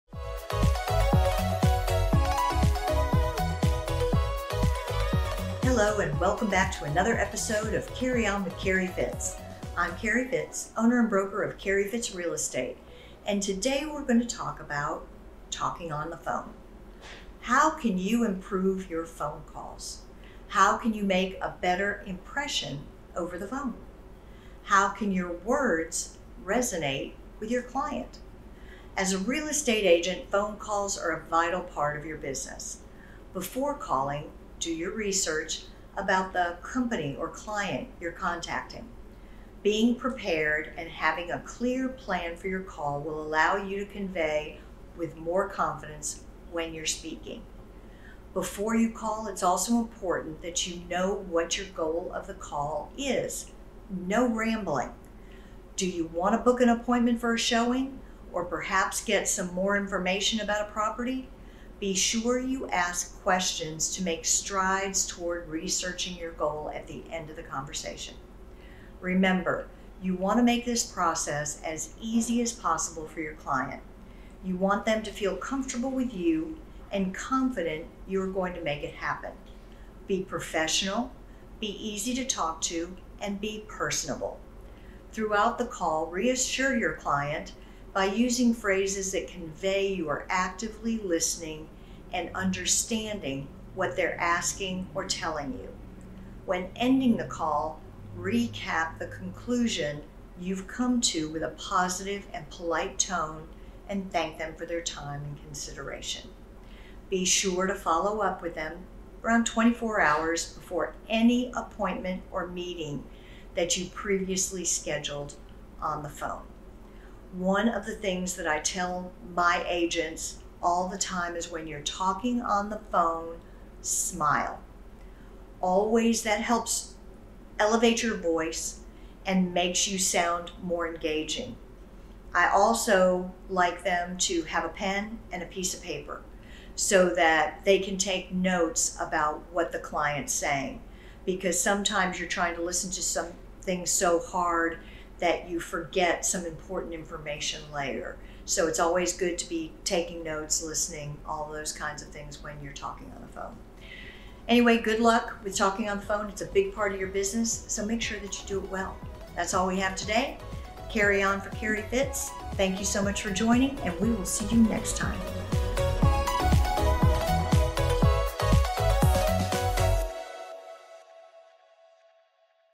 Carry On Carrie Fitts | Episode 44 | Talking on the Phone
If talking on the phone is a big part of your business, make sure you do it well! How can you improve your phone calls & make a better impression?